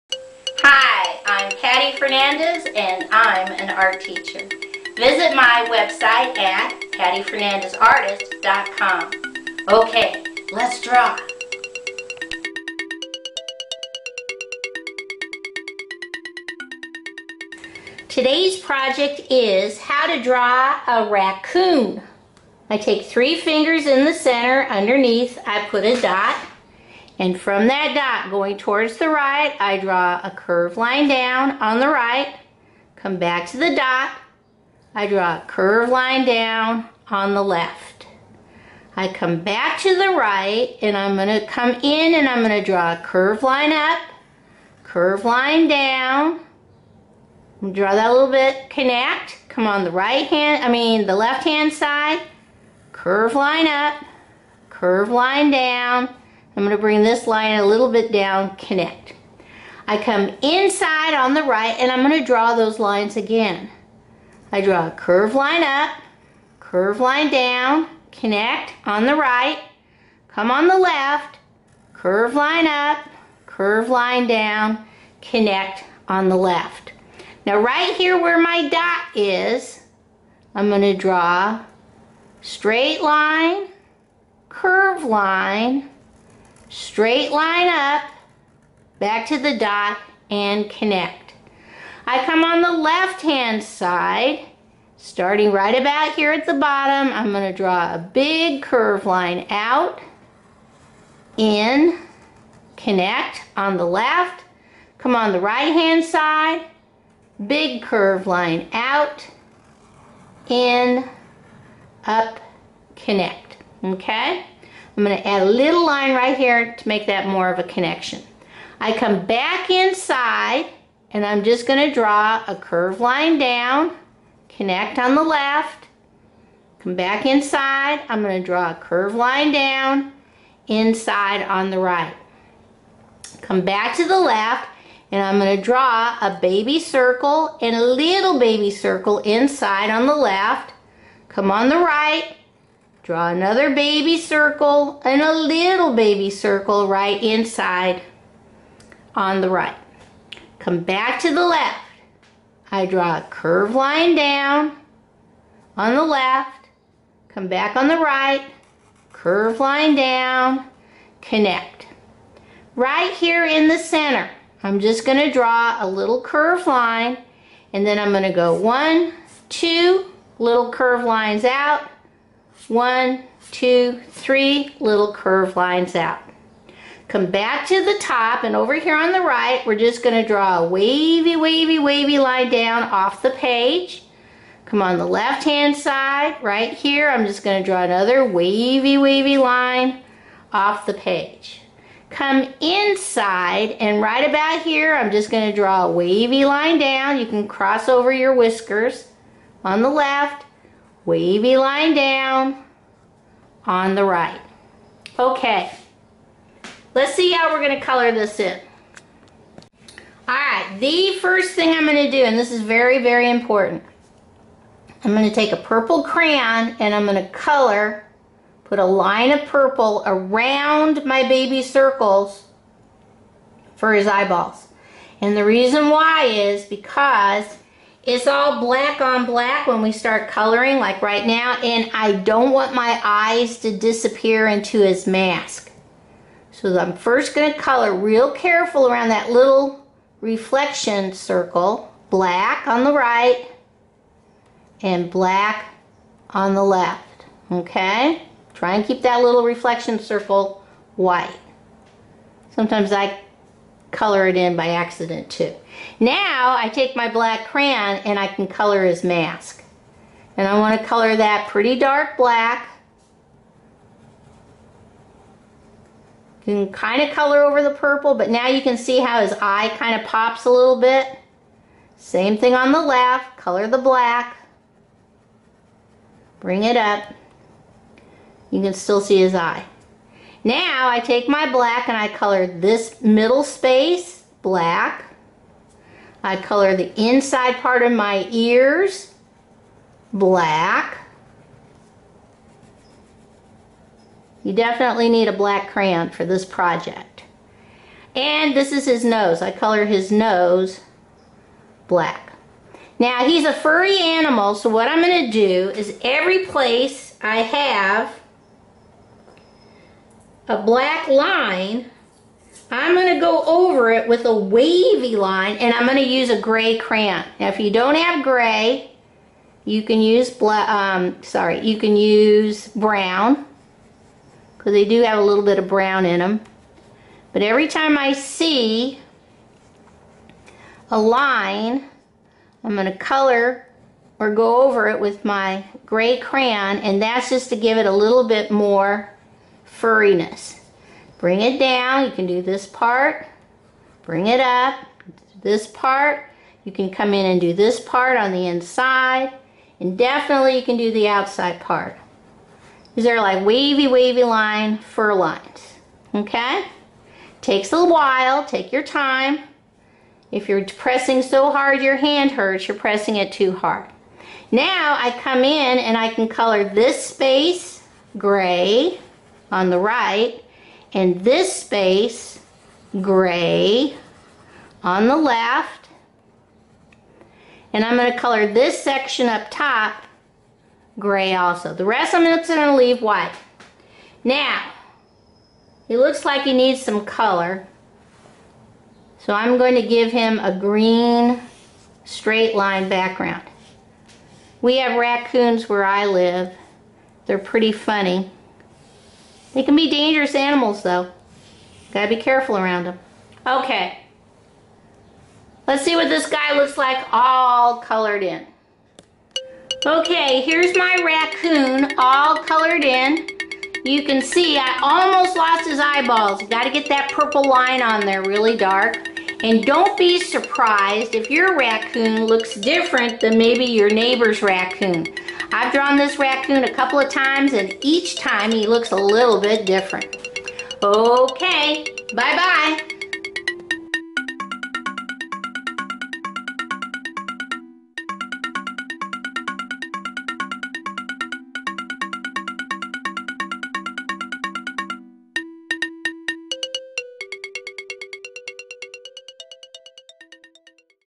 Let's Draw a Raccoon!
Easy to follow directions, using right brain drawing techniques, showing how to draw a Raccoon.